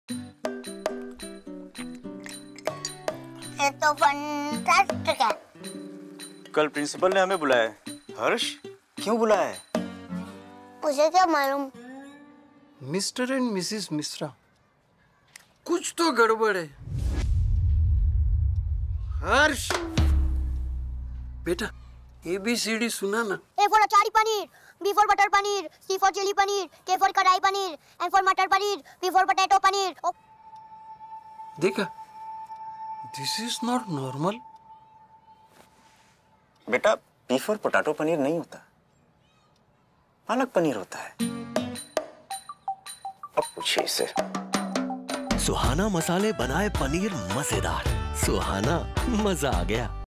Suhana Masalon se bane Paneer Mazedaar | Suhana Paneer Mixes | Suhana TVC | Suhana Masala
Suhana Masalon se bane Paneer Mazedaar!
Kuch toh gadbad hai! 😅 Chahe Principal ho ya Pappa, Sabka dil pighlega with Suhana Paneer Masala.
Paneer ka swaad aur Suhana Masala ka saath 🤝😋

सुहाना मसालों के साथ पनीर बने मजेदार!
कुछ तो गडबड है! 😅प्रिंसिपल हो या पापा, सब का दिल पिघलेगा सुहाना पनीर मसालों से। पनीर का स्वाद और सुहाना मसालों का साथ।🤝😋